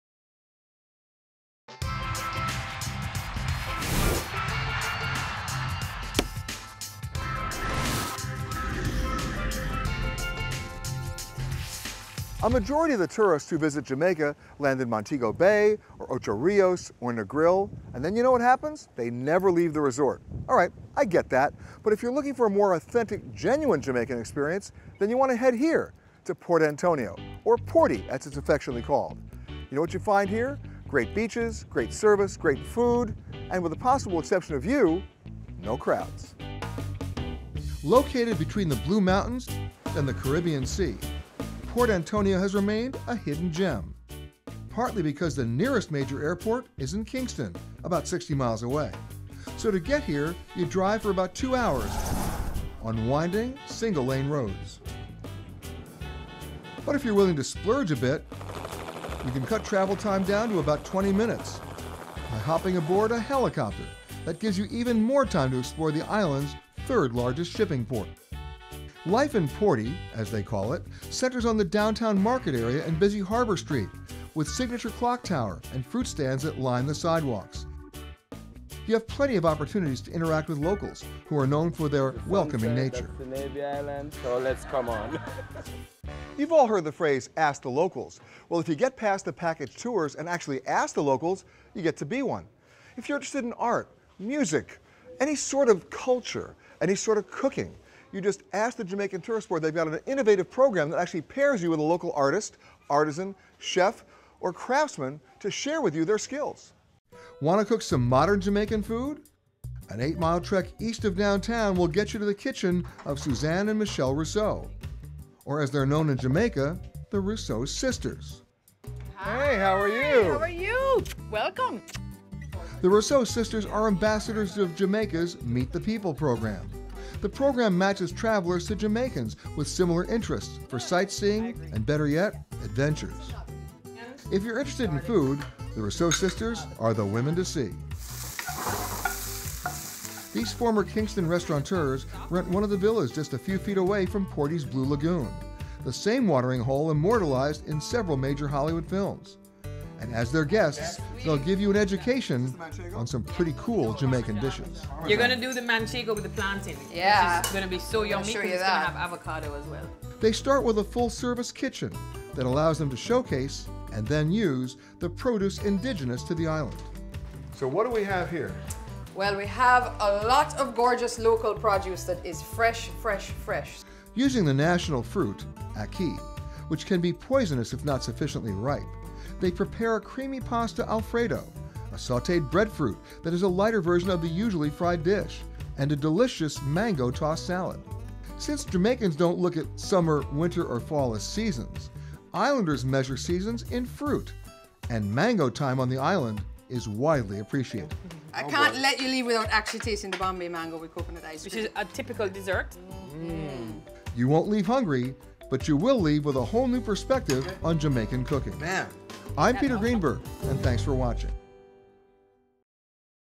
Meet the People: Hidden Gems of Jamaica
A majority of the tourists that come to Jamaica end up landing in Montego Bay or Ocho Rios or Negril and then you know what happens? And they may never leave the resort. But if you want to experience Jamaica, like the locals. Then you've got to meet the people.

In Port Antonio or "Porty," you'll find great beaches, great service, great food and no crowds. But that's just the start. If you're interested in art, music, any sort of culture-any sort of cooking-you just ask the Jamaican tourism board. They have an innovative program that actually pairs you with a local artist, artisan, chef or craftsman to share with you their skill.

Find out more in Peter Greenberg's last Hidden Gems of Jamaica.